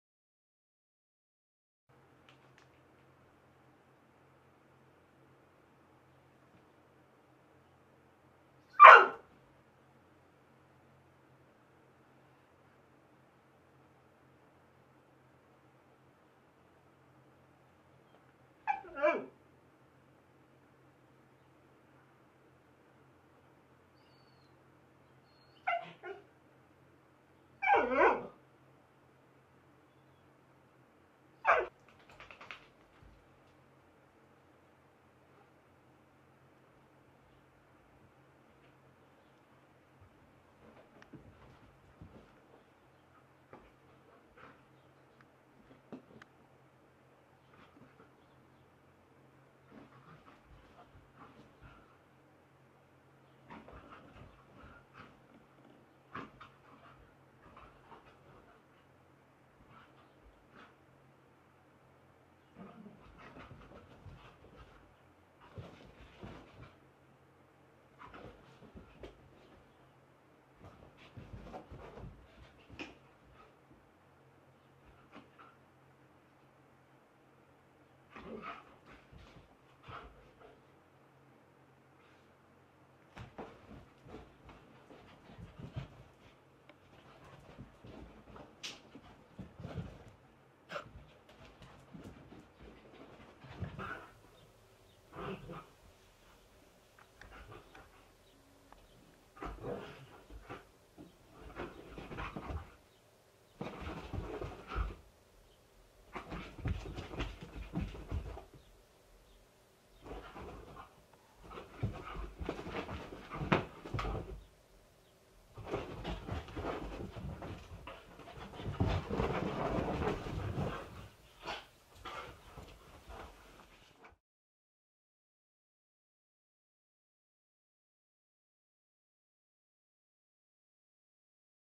Harley Spins-Nikki Documentary 01-H.264 - Webcasting .mov
Greyhound Harley enjoys retirement with Harmony looking on..